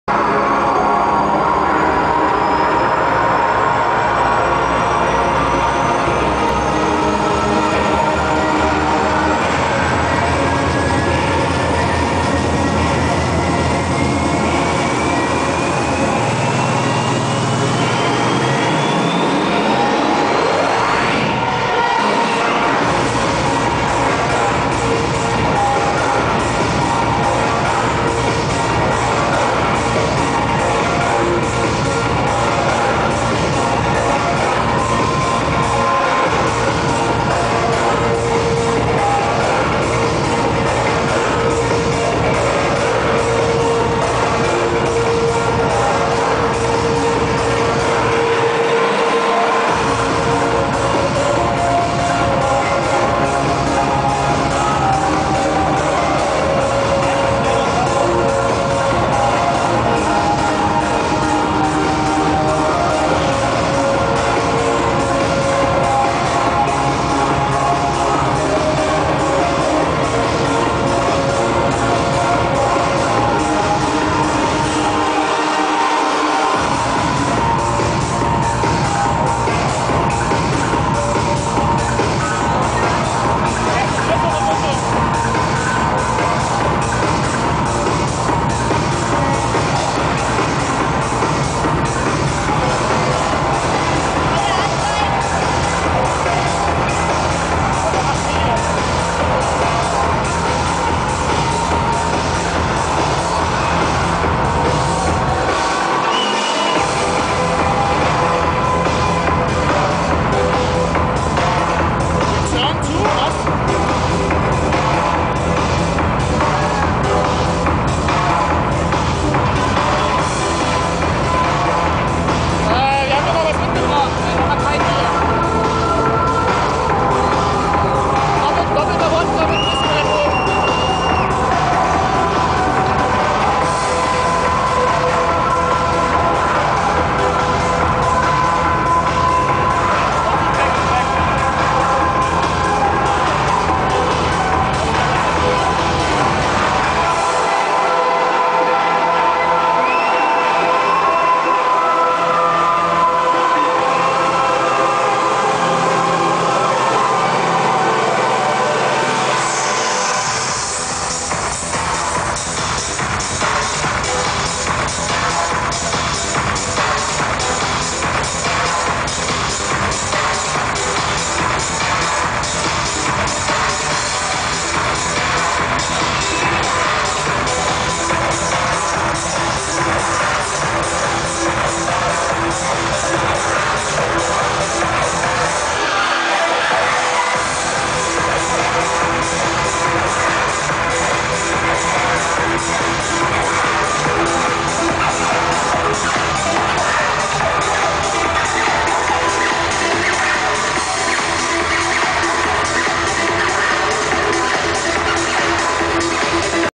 A Gig at Fullmoonfestival at Czech Republik in February 2010...in an old Cinama, nice Location...strange Party.
you see Dj Chris-a-Nova and dancing People.